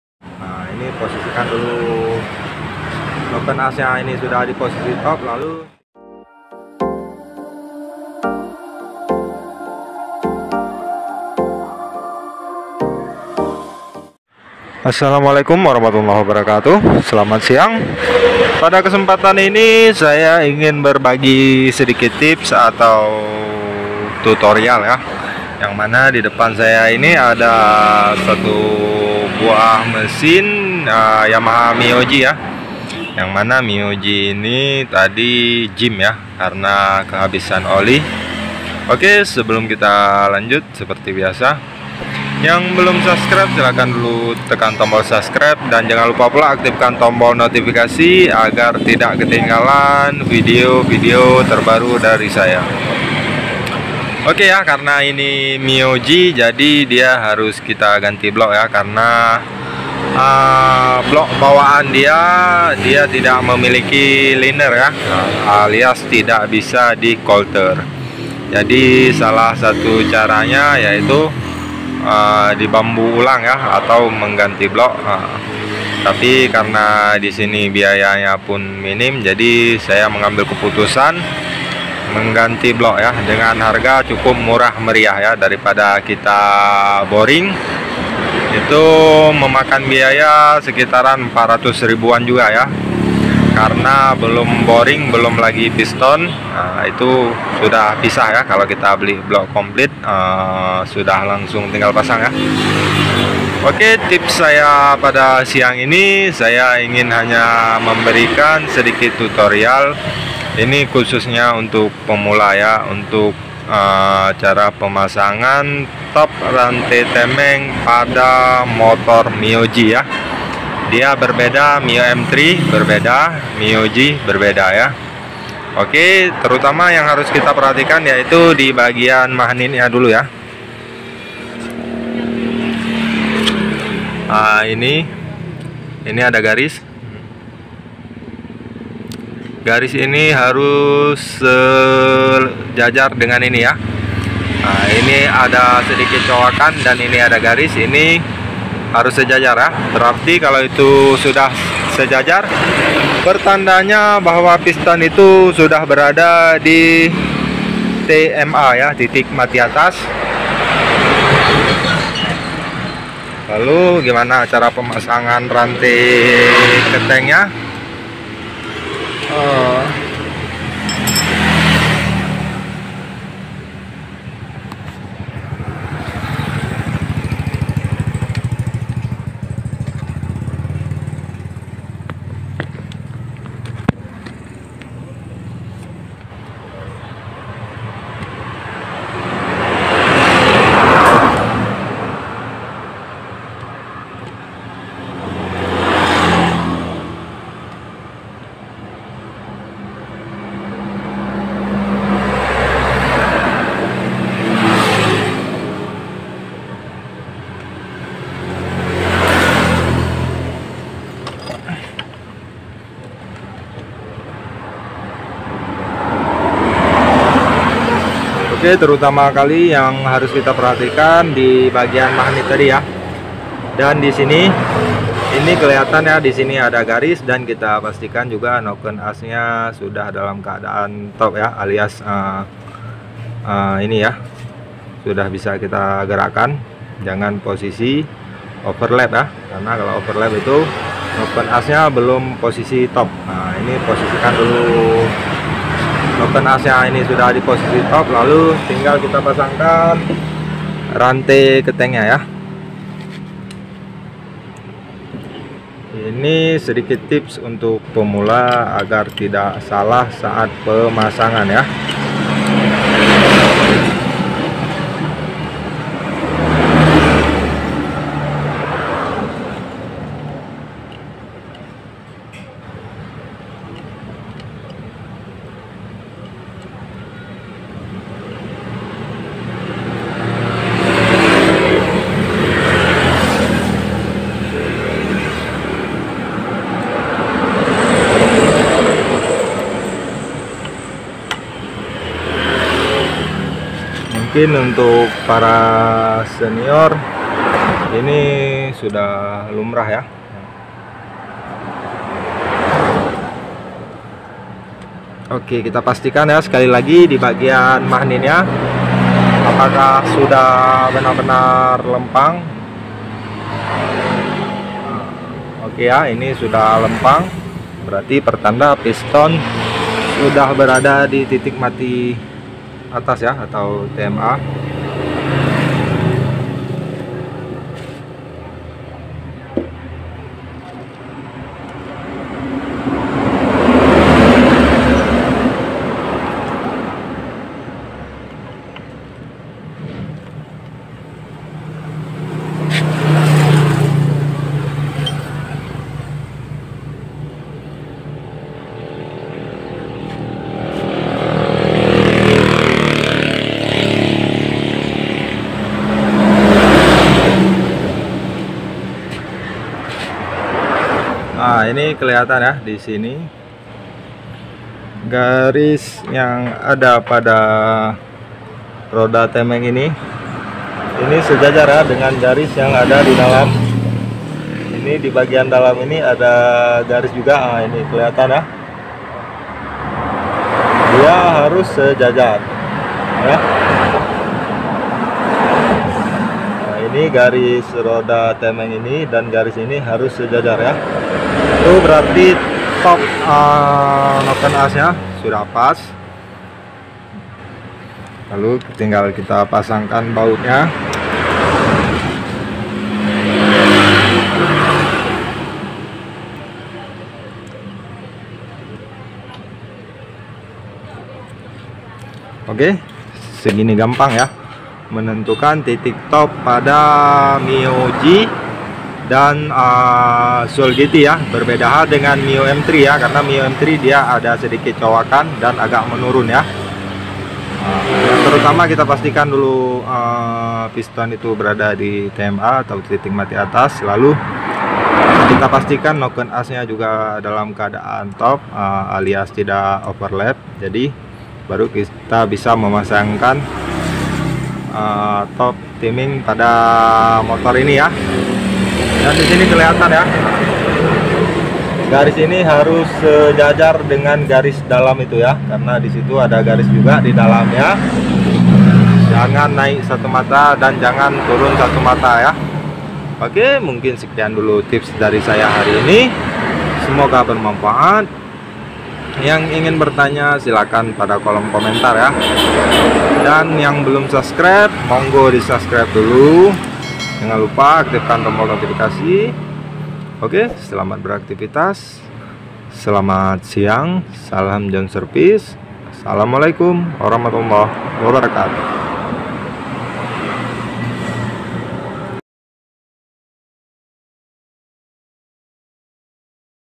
Harus Bisa Pemula! Cara Menentukan Titik Top Noken As Mio J
Cara Menentukan Titik Top Noken As Mio J
Simak Vidio Ini
Salam Jhon Service